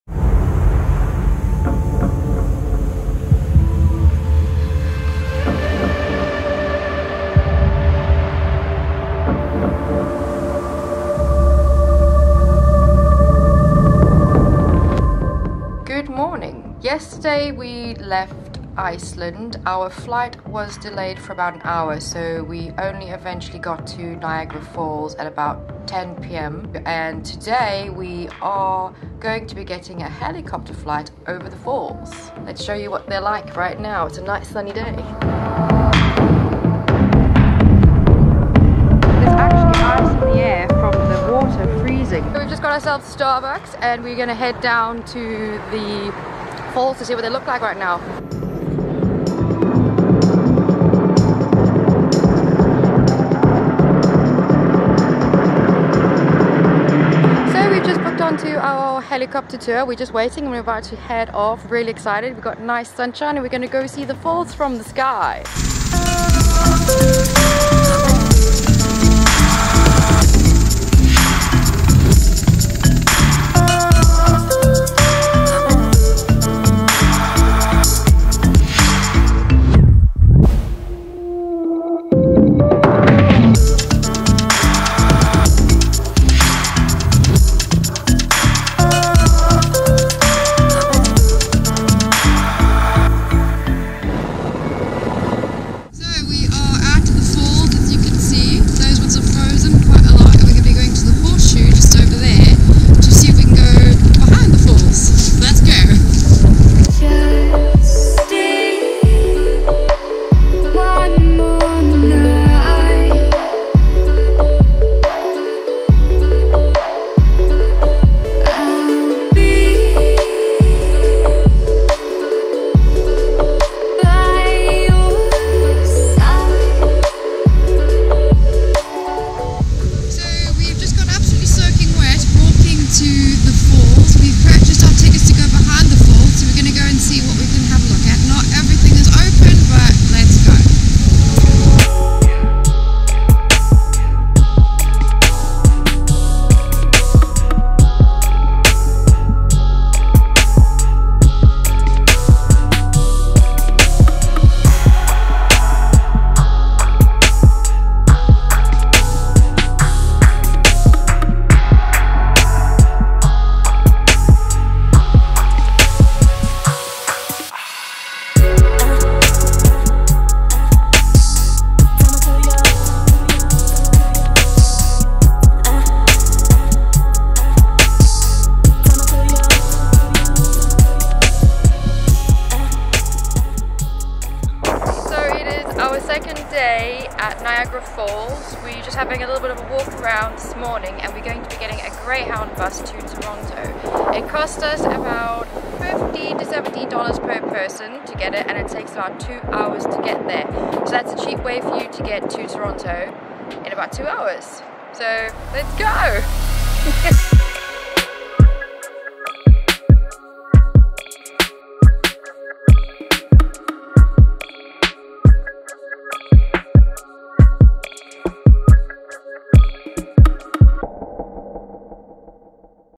Niagara Falls
Niagara Falls in the winter is beautiful. In February 2018  Mather Brown Travel takes you on a helicopter tour over the Falls to see just how spectacular Niagara is from the sky.
 The cheapest price online that we found was from (to niagara.com)
which was roughly about CA$127 per person.
Journey Behind the falls in winter is limited but worth going down to see.
When visiting the falls in winter, make sure you cover up well as the mist that blows up from the falls instantly freezes and you end up all frosty and frozen.